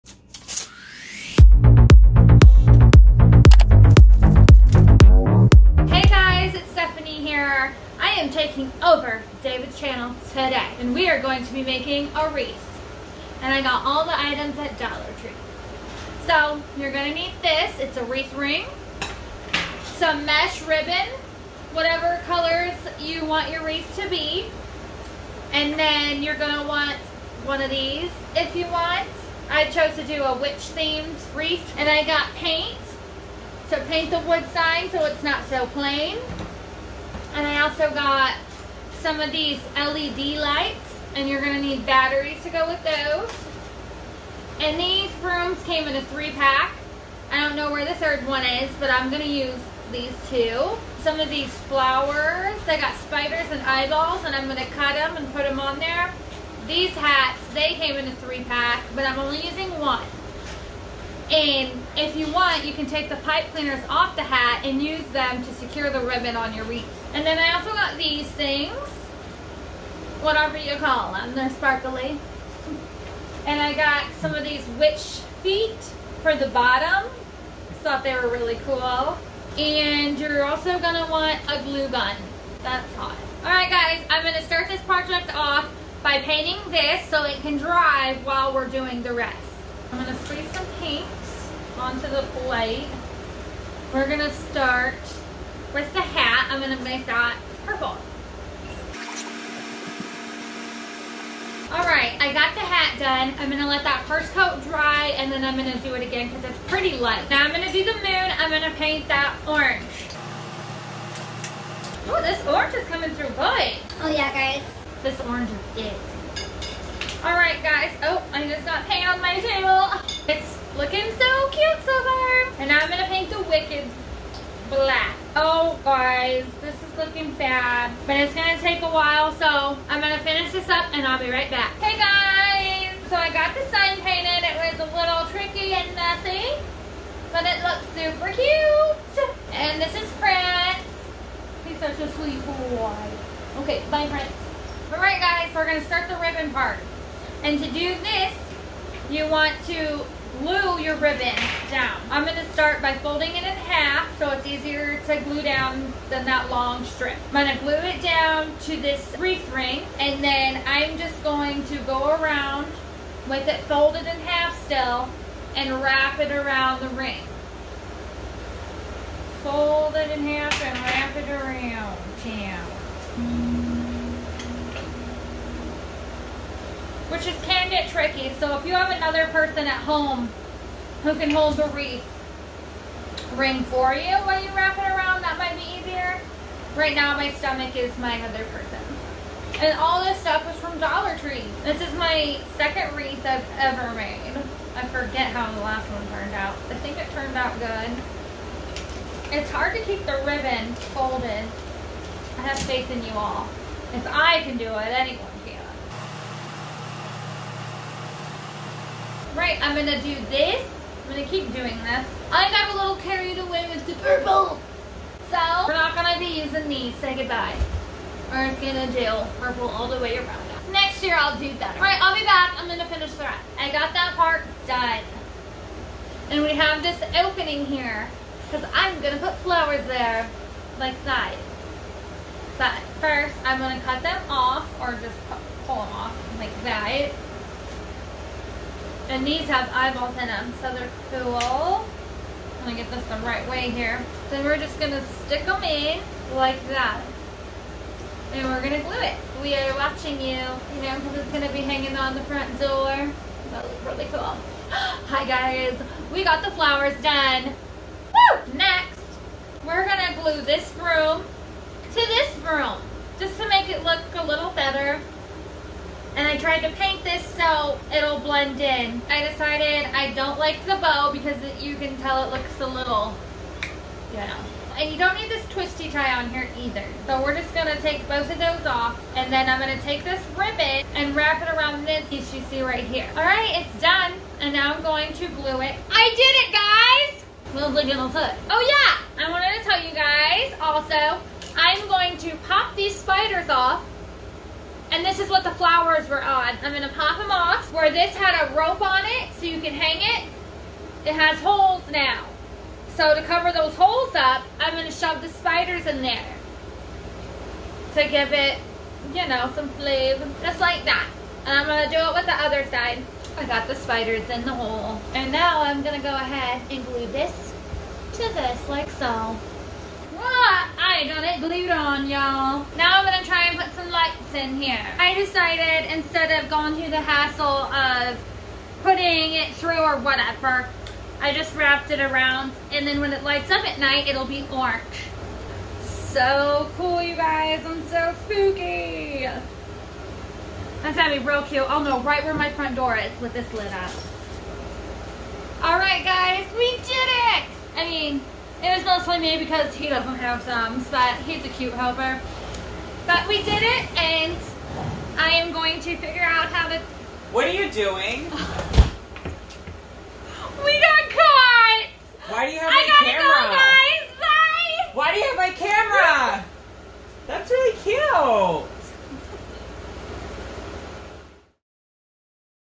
DIY DOLLAR TREE HALLOWEEN WREATH
someone took over my youtube channel. whomever it is.. made a GORGEOUS Halloween wreath for 2020.